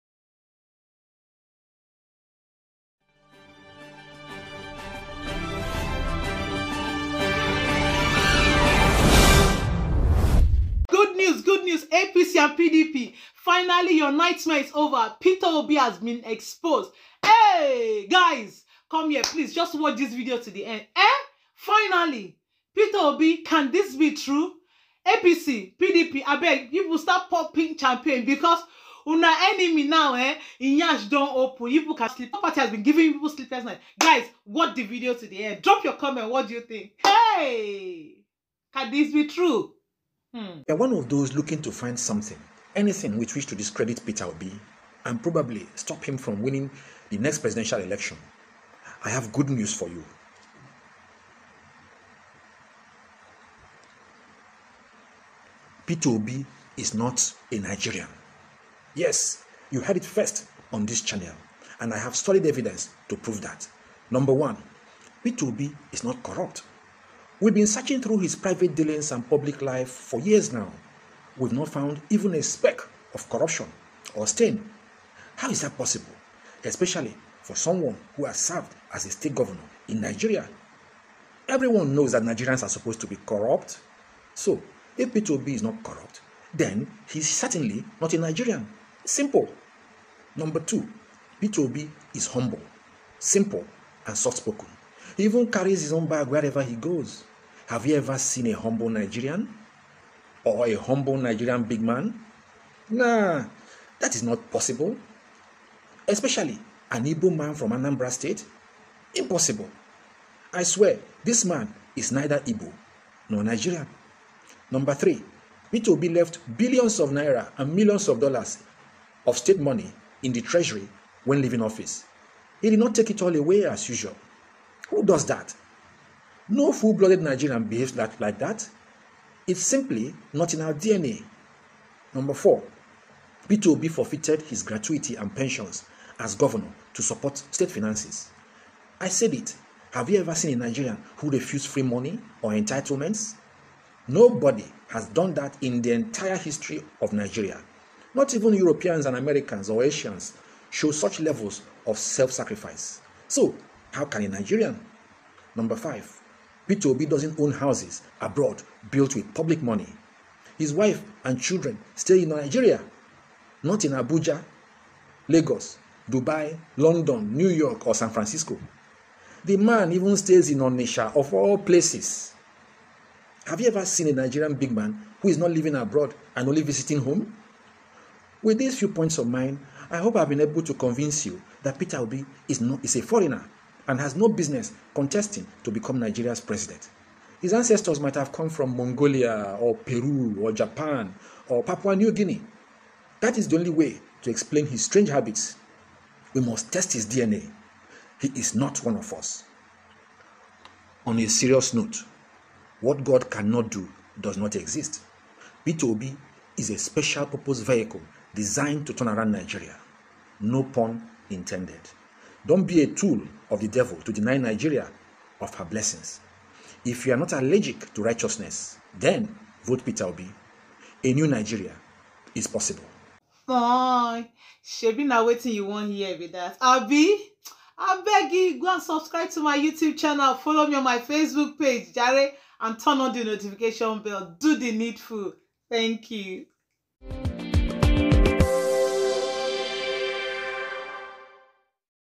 BREAKING NEWS: PETER OBI IS EXPOSED.
Peter Obi The Labour party Presidential Candidate Is exposed.

Powered by: The Elipses Ideation company.